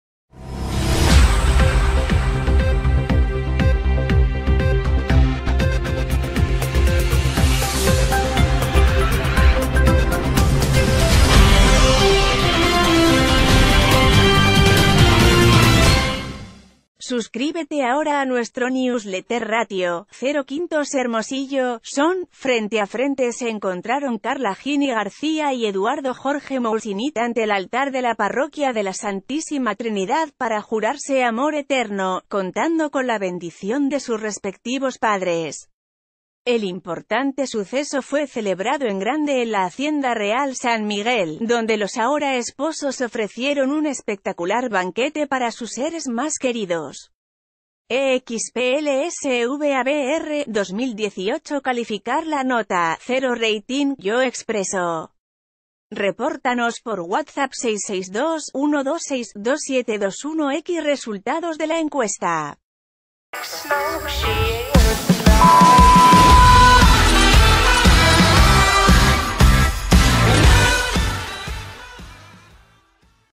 Son el uno para el otro
Suscríbete ahora a nuestro newsletter Ratio: 0 / 5 HERMOSILLO, SON.- Frente a frente se encontraron Karla Heaney García y Eduardo Jorge Mawcinitt ante el altar de la Parroquia de la Santísima Trinidad para jurarse amor eterno, contando con la bendición de sus respectivos padres. El importante suceso...

{hashtag}